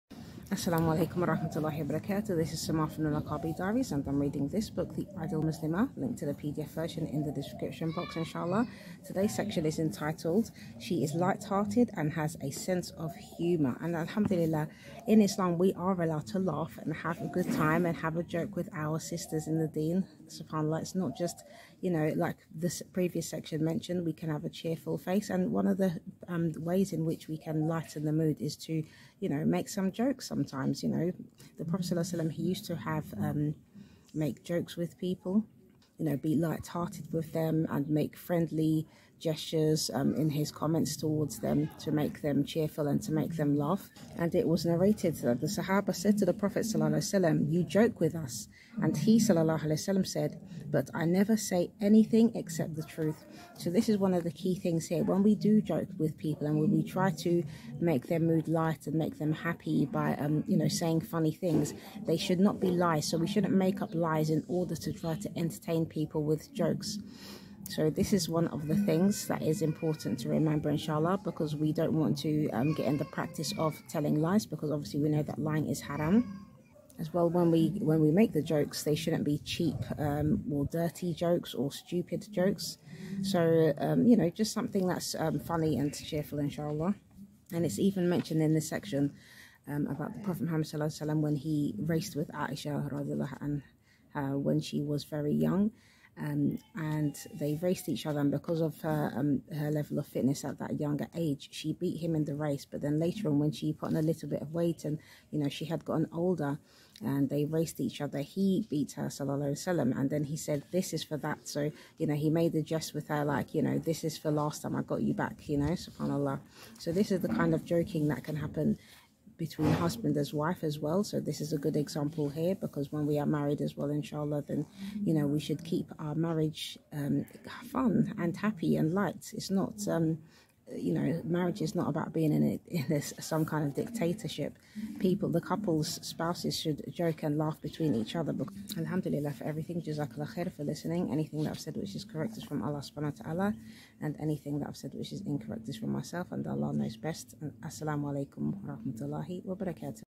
Reading The Ideal Muslimah part 156- She is lighthearted and has a sense of humour
Assalamu Alaykum, If you want to read this book with me you can download the pdf directly here:
Its an automatic download when you click the link.